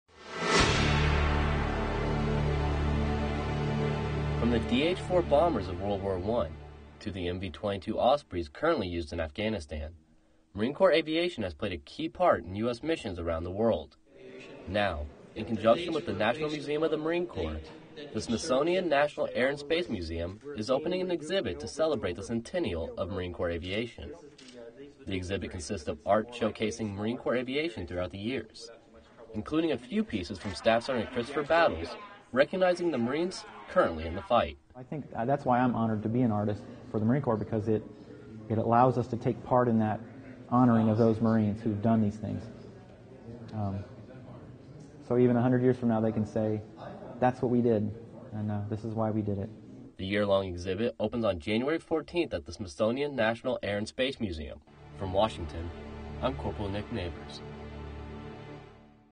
Celebrating 100 years of Marine Corps Aviation
Since Marine Lieutenant Alfred A. Cunningham reported to Naval Aviation Camp in May 1912, the ability to attack from the sky has played an invaluable role in the war fighting capabilities of the Marine Corps. Cpl. Nick Neighbors shows us a new exhibit aimed at memorializing Marine Corps Aviation.